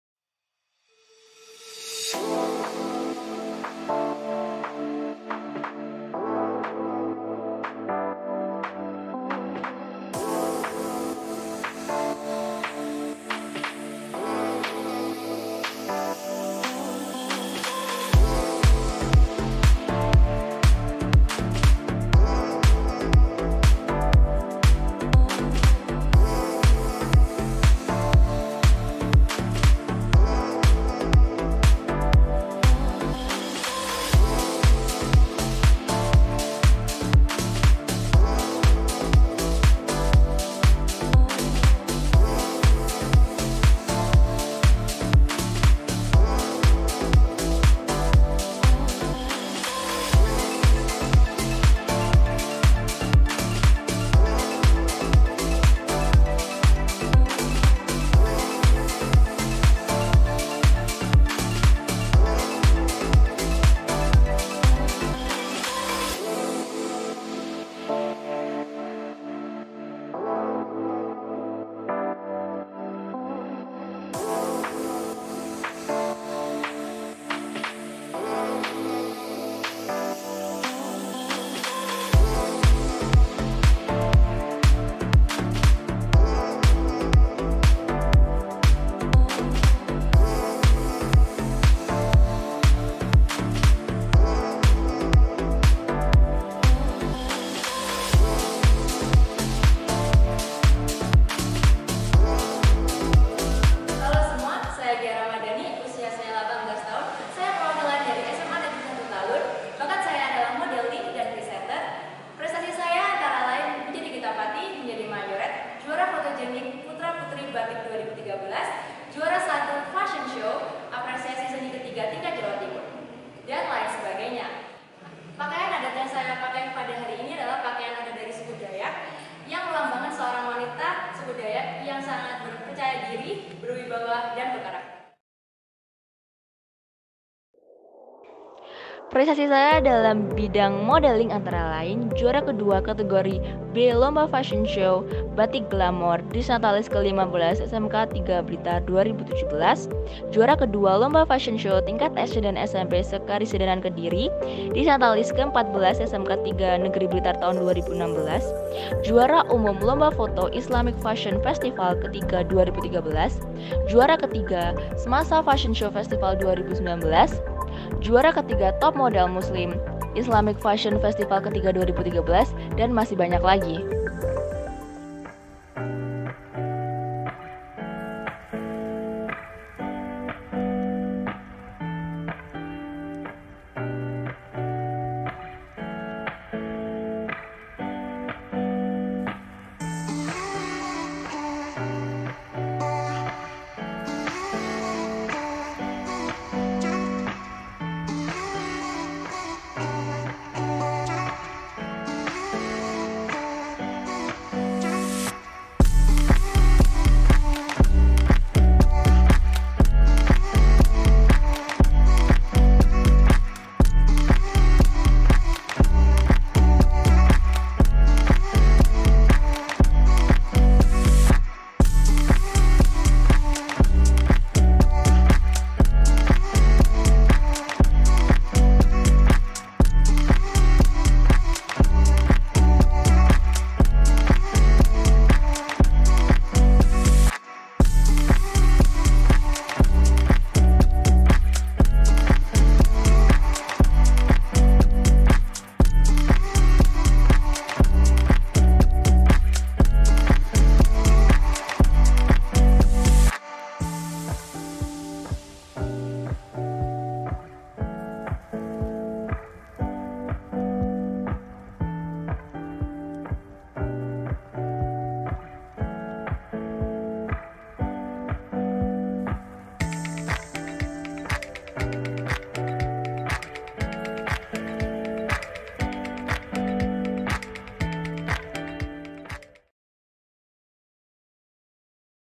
Jawa Pos SMA Awards 2021 - Modeling - SMA NEGERI 1 TALUN
SMA, MAJU BERSAMA HEBAT SEMUA!!!
SMANTA JAYA!!!

Modeling adalah meniru perilaku dan sikap orang lain, di mana orang yang di modelkan merupakan suatu pola untuk dapat ditiru. Pola yang memberikan dorongan untuk menjadi perilaku ke arah yang lebih baik dari sebelumnya. Model yang di gunakan memiliki kesan bagi observer yang melihatnya.

Saya Gea Rachmadani
dari SMA Negeri 1 Talun
siap mengikuti Jawa Pos SMA Awards 2021
Cabang Lomba Modeling

SMANTA JAYA!!!